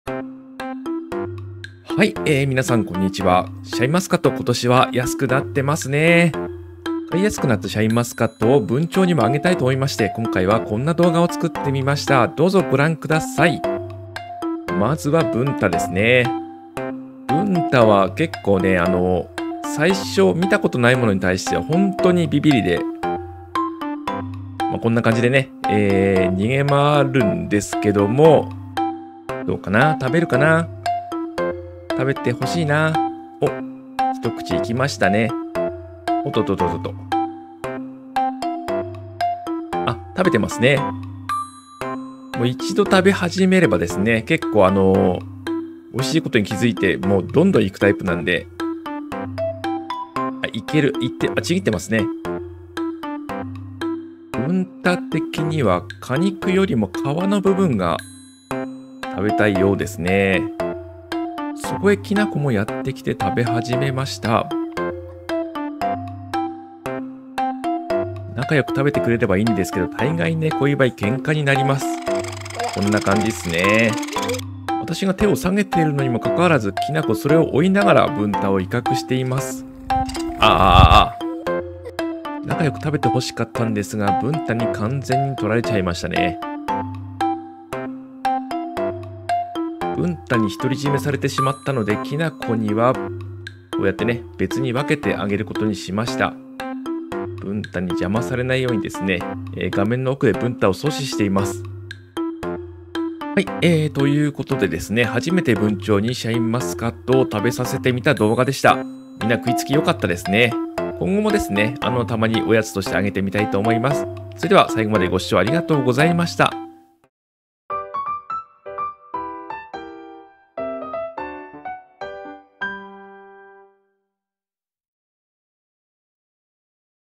文鳥にシャインマスカットをあげてみた！
今回の動画は、文鳥にはじめてシャインマスカットをあげてみたという内容です。